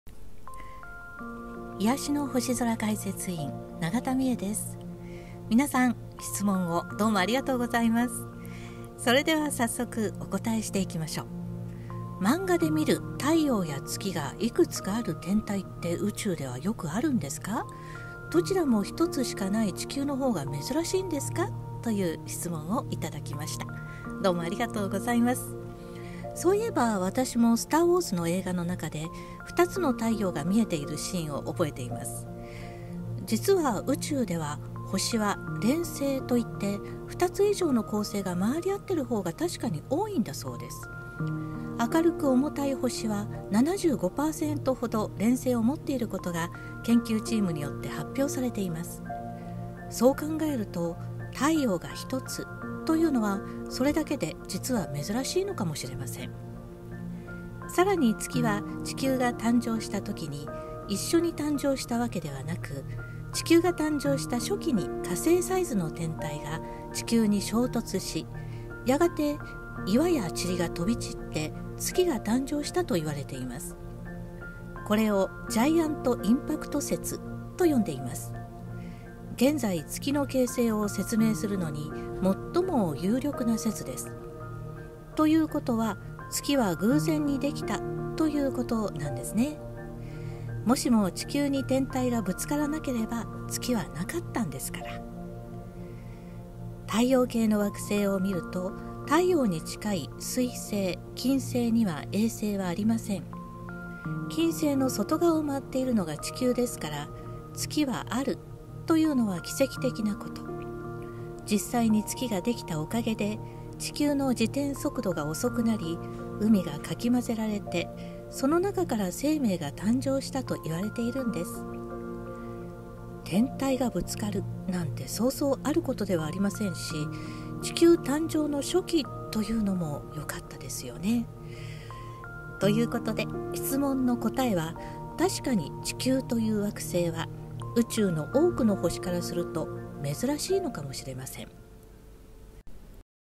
CosmoRadio -質問回答①太陽と月-
動画設定の画質から1080pに設定してご視聴いただくことをおすすめします

太陽と月がひとつずつある、そんな地球は珍しい存在なのでしょうか？
今回の動画から視聴者のみなさまからいただいたご質問にお答えしていきます。
たくさんのご質問をありがとうございました。宇宙の不思議を、どうぞお楽しみください。

▼解説員
永田美絵さん

▼制作
株式会社アストロアーツ

▼シミュレーションソフトウェア
ステラナビゲータ11

▼アストロアーツ公式ブログで「CosmoRadio制作の裏側」連載中！